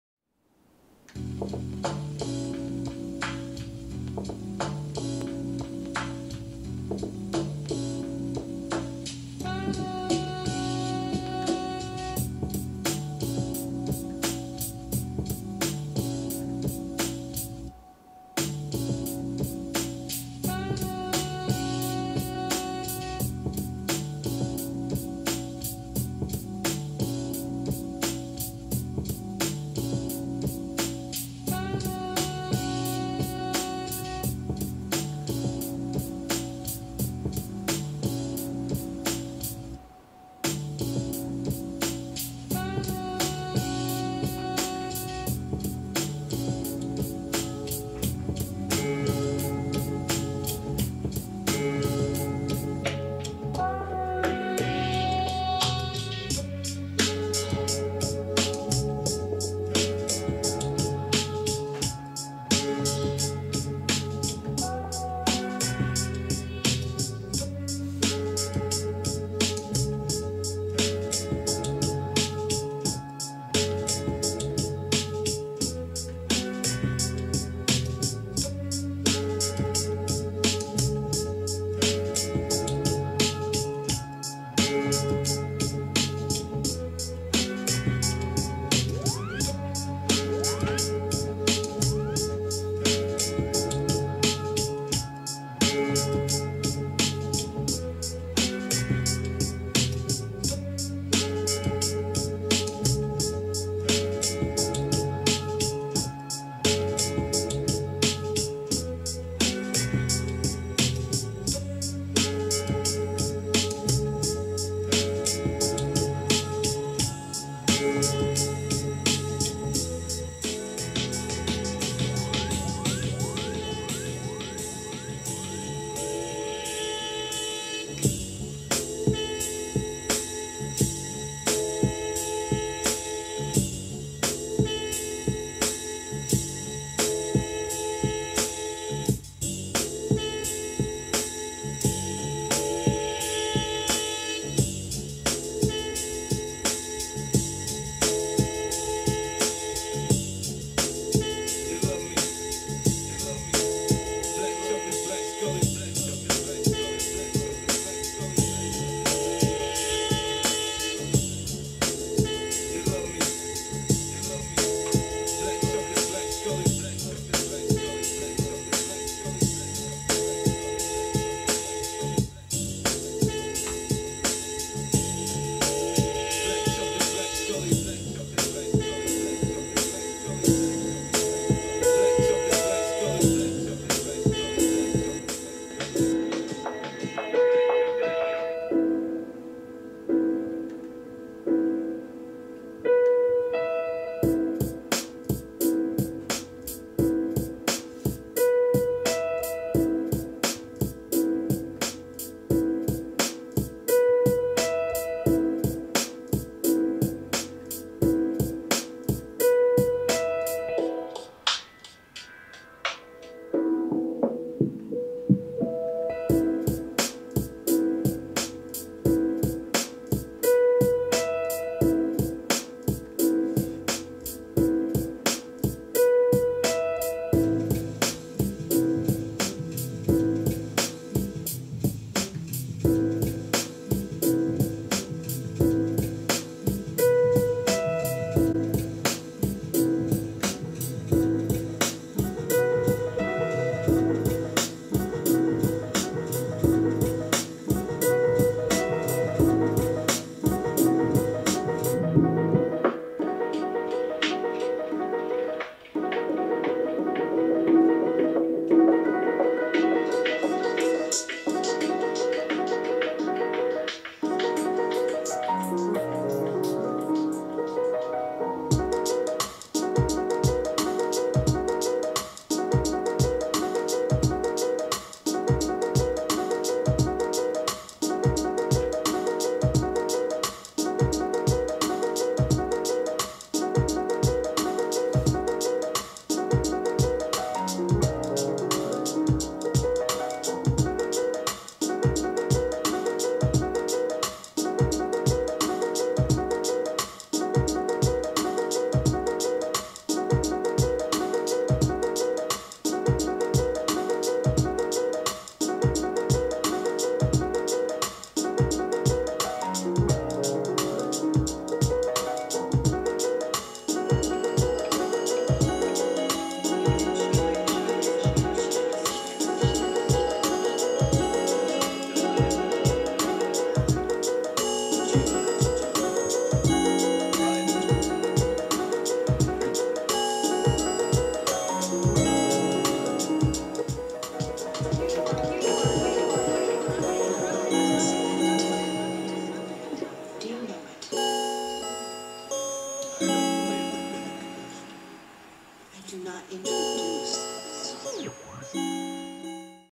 Henry Kissinger x Pelican Magazine - Words + Music
Local producer Henry Kissinger offers up a delicious mix of beats, accompanied by words and stories about his life with music.